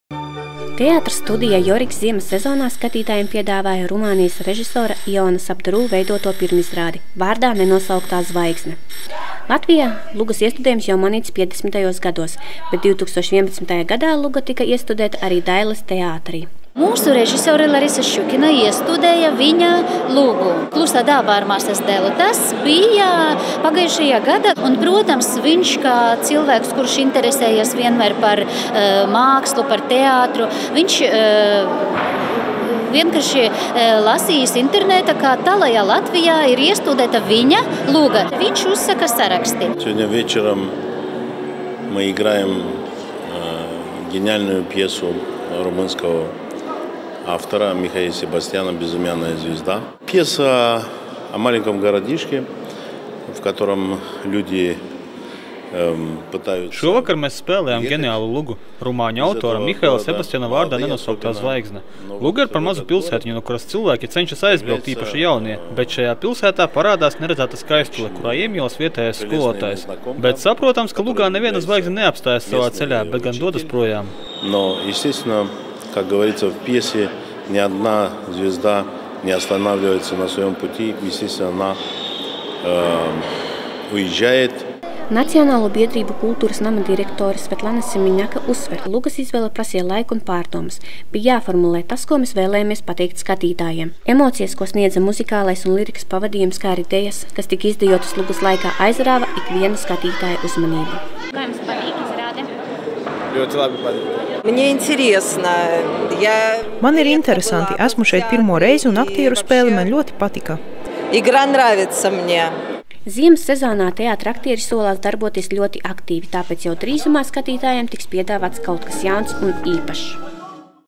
Teātra izrāde - “Vārdā nenosauktā zvaigzne”
Decembra mēnesī teātra studijā - Joriks bija iespēja vērot Rumānijas režisora Iona Sapdaru veidoto pirmizrādi “Vārdā nenosauktā zvaigzne”. Šī luga Latvijā tika spēlēta vēl piecdesmitajos gados, bet
bet 2011. gadā iestudēta Dailes teātrī.